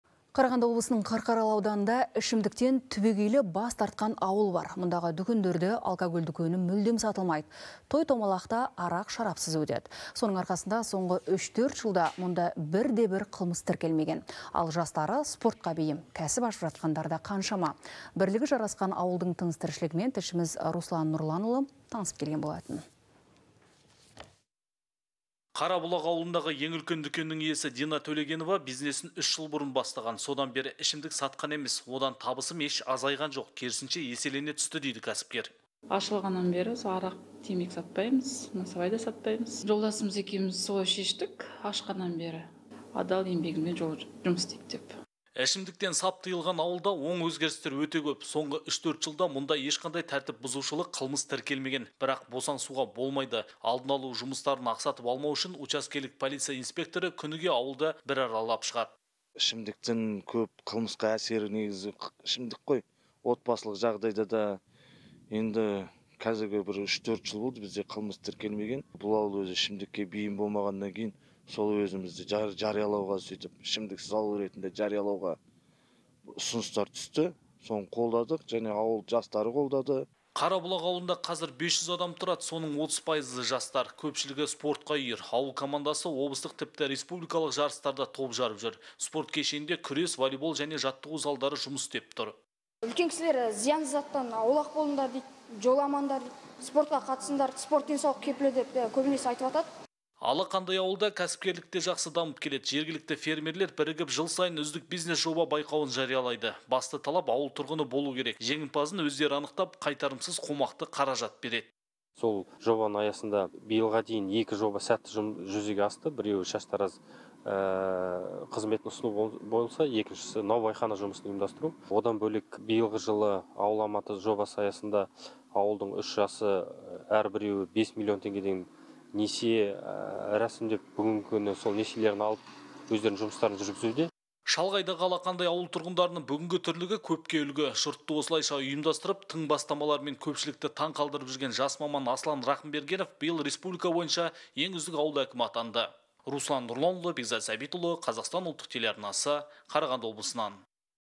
Қарағанды облысында ішімдіктен түбегейлі бас тартқан ауыл бар
Мұндағы дүкендерде алкогольдік өнім мүлдем сатылмайды. Той-томалақ та арақ-шарапсыз өтеді.